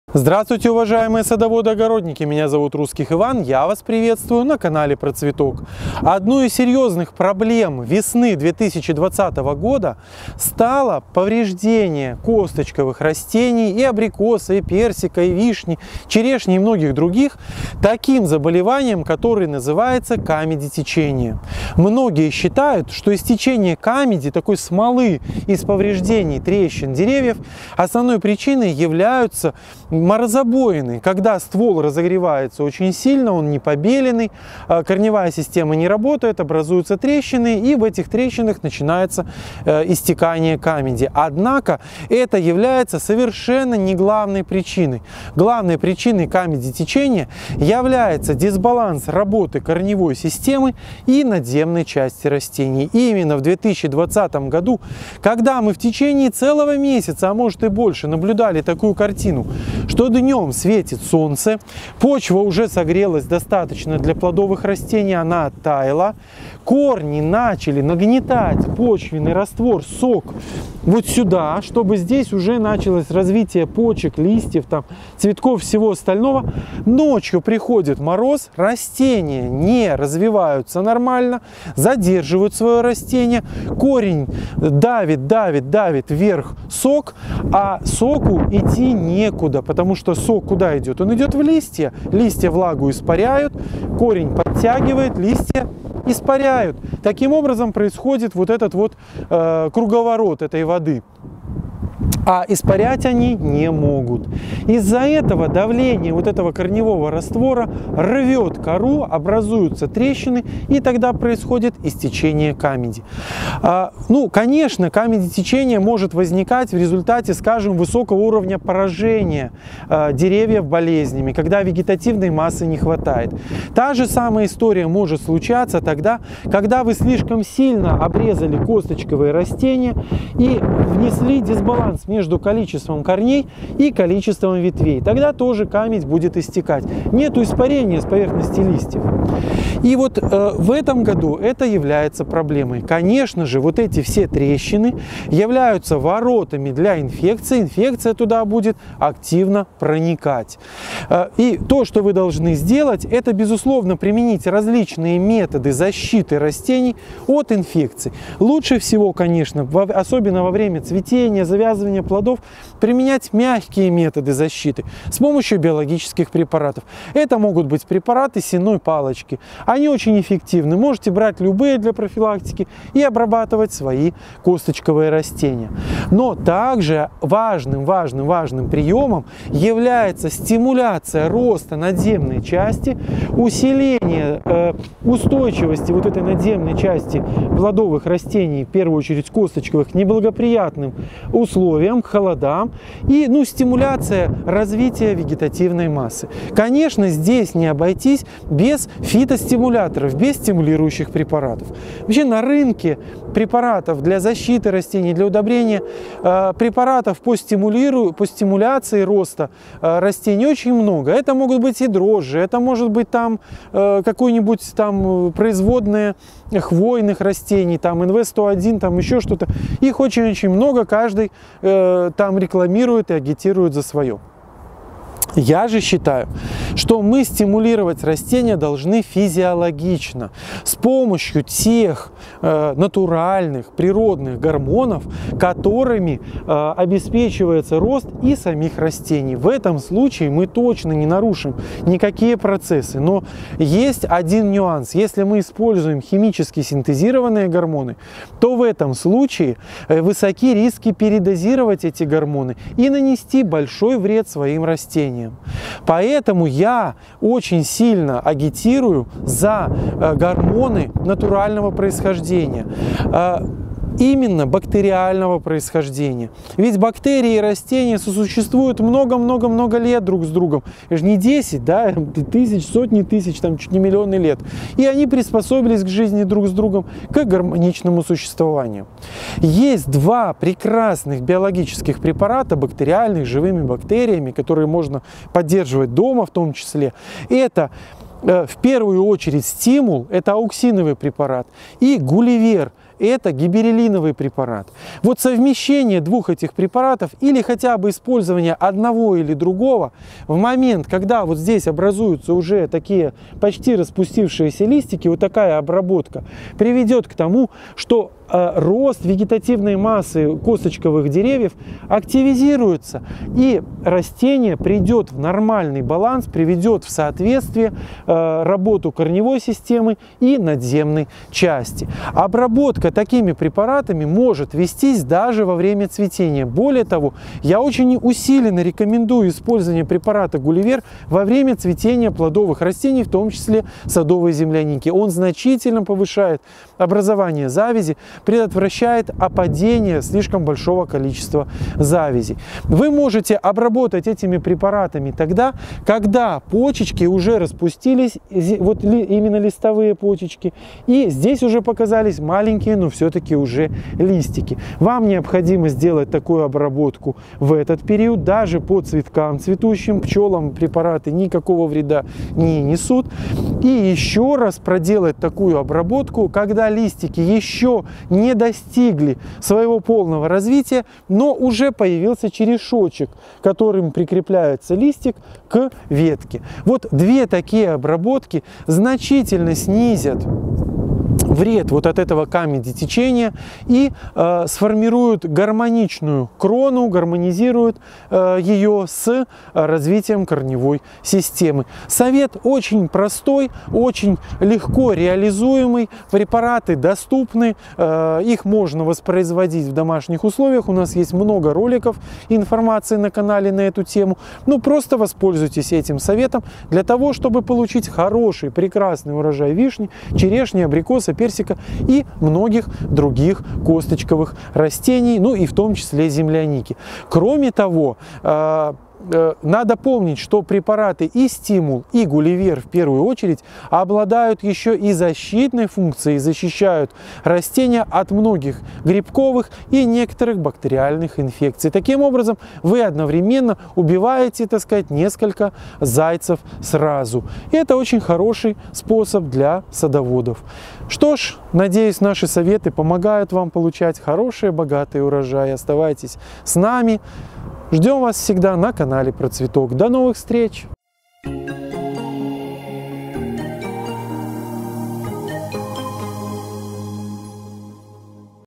КАМЕДЕТЕЧЕНИЕ КОСТОЧКОВЫХ. Как вылечить деревья и при этом увеличить их урожайность?
✿Всё для сада и огорода ищите в нашем интернет-магазине!

Камедетечение (или гоммоз) - распространенное заболевание косточковых деревьев, таких как абрикос, персик, черешня, вишня и др. С чем связано камедетечением плодовых? Какие есть меры профилактики и лечения камедетечения? Обо всем этом я расскажу вам в новом видео.